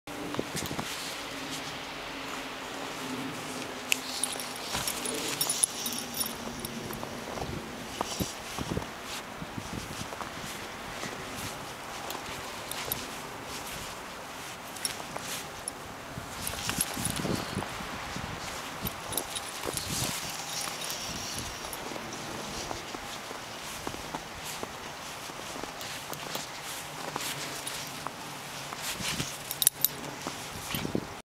Gizmo the Japanese Chin
Gizmo likes to play around in the snow!